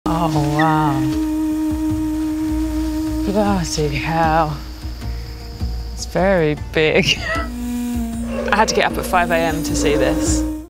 Okayyyy, let's go
Writer and comedian Sara Pascoe returns with her unique travelogue, learning how to do the world’s endangered jobs.